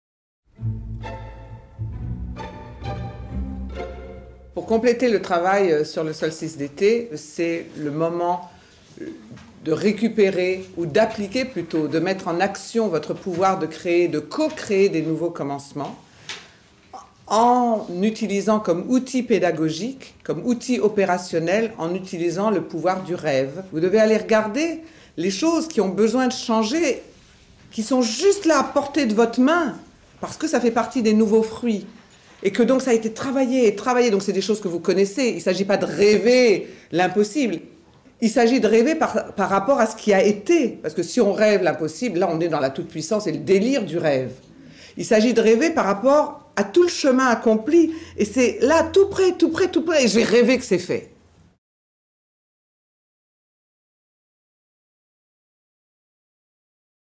De la Saint Michel à la Saint Jean - la voie du plaisir
Des bonnes résolutions de la rentrée aux fruits de fin d'année, le solstice d'été clôture la roue annuelle des saisons.
Une conférence pour finir l'année en beauté, rêver à plus de plaisir et faire du feu de la Saint Jean un feu de joie.